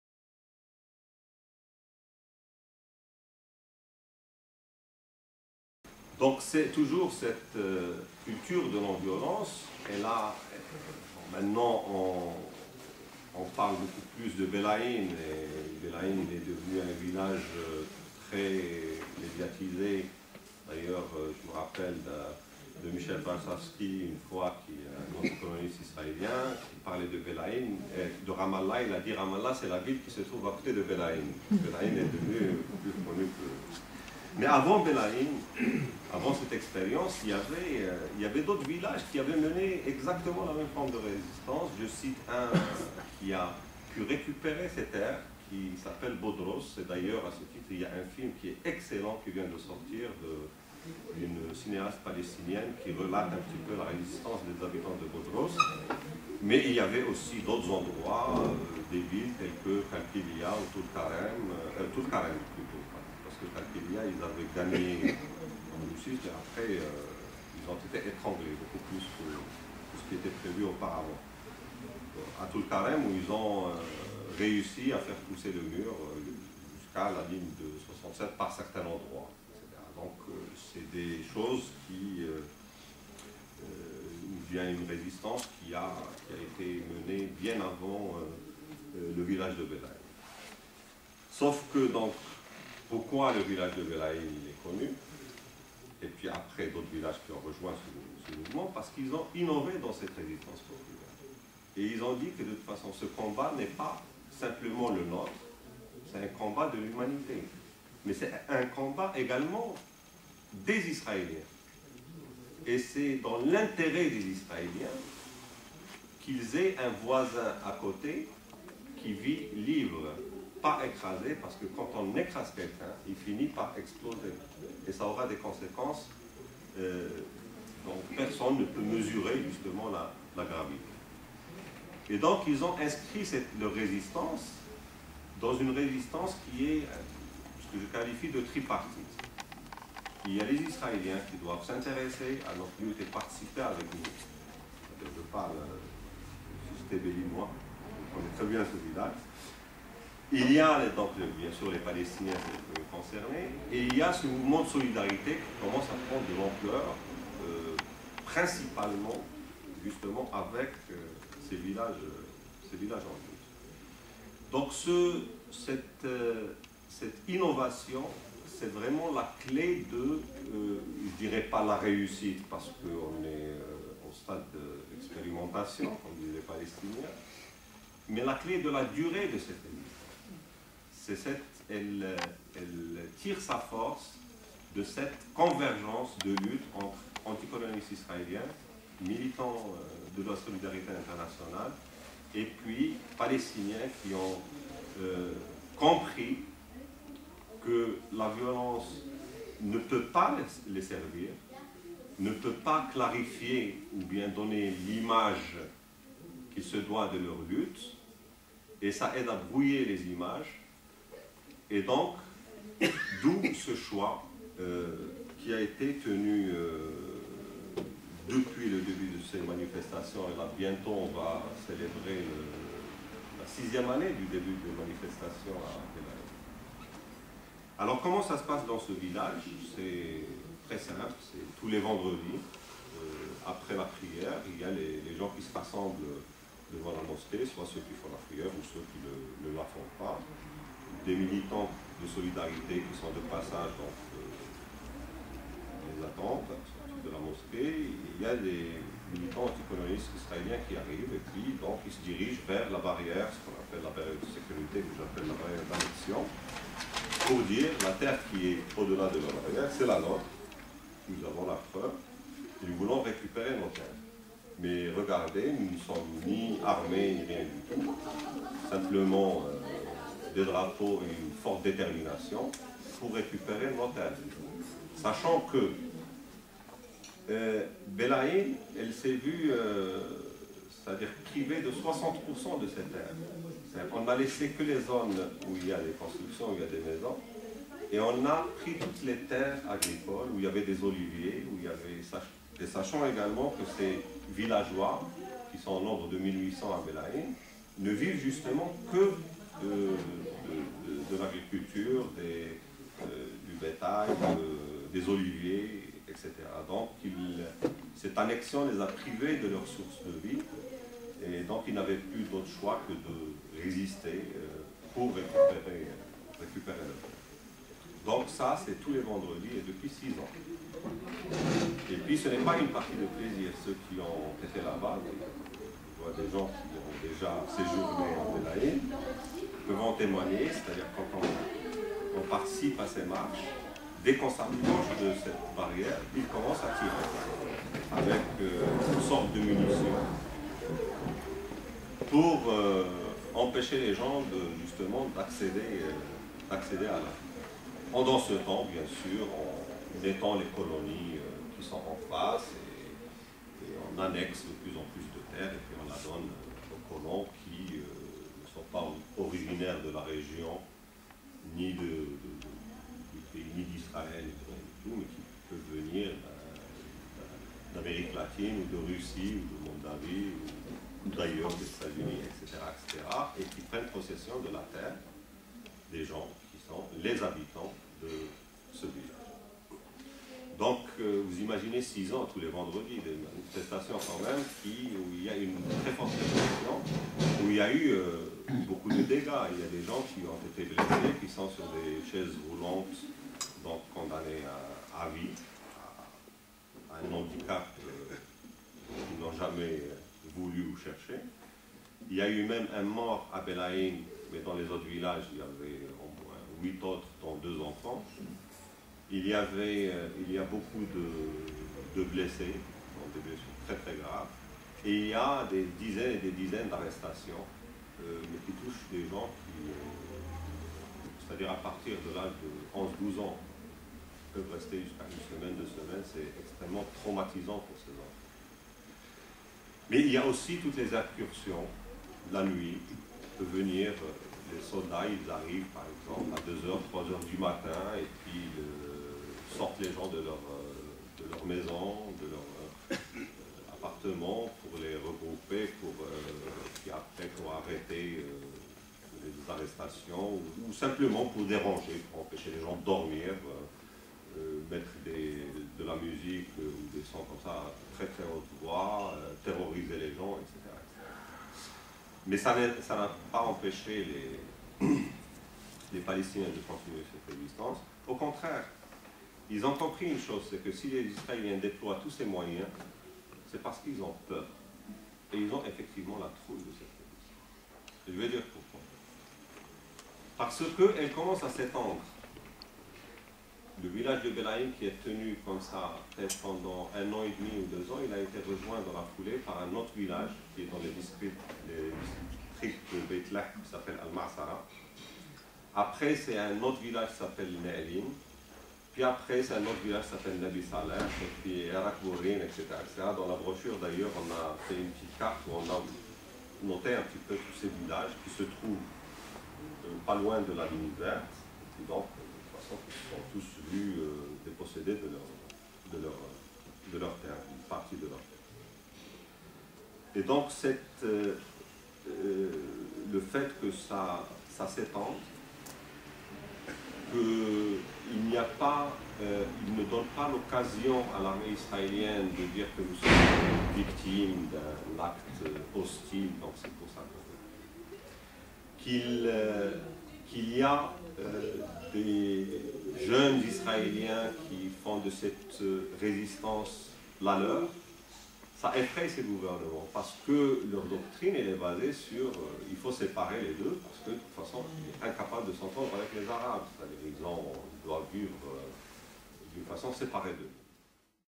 Débat avec Taoufiq TAHANI Dijon ... 2
Débat
avec
la participation
de
Taoufiq TAHANI
à l'initiative
du MAN et des différentes associations
du Collectif Solidarité Palestine 21
01/10/2010
Dijon